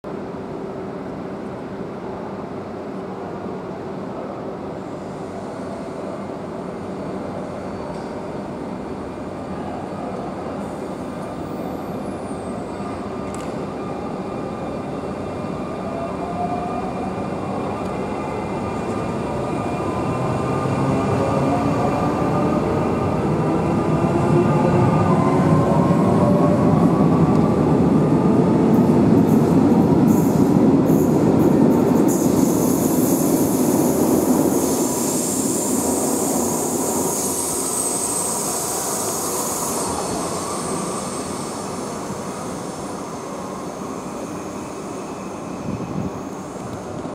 NMBS MS96 553 vertrekt van station Liège-Guillemins
Dit is de Intercity naar Liers via Liège-Jonfosse, Liège-Palais, Herstal en Milmort,